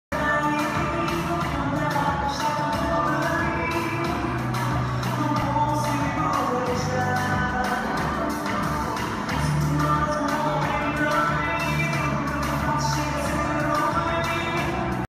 Kizomba demo - Aladje & Elena - DanceOptions Studio 16.03.2016
Уроки Кизомбы в Москве с Aladje & Elena. ☎️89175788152